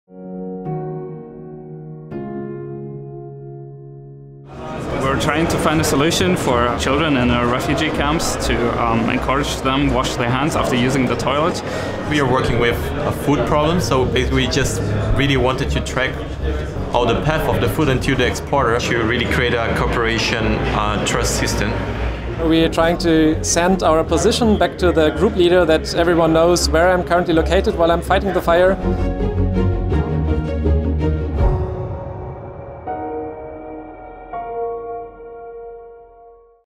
Bosch ConnectedExperience 2019 – Hacking results & prototypes of Bosch’s IoT hackathon
On May 13-15, more than 700 developers got down to work in teams to tackle various hack challenges in selected user domains. Learn more about the ideas they came up with.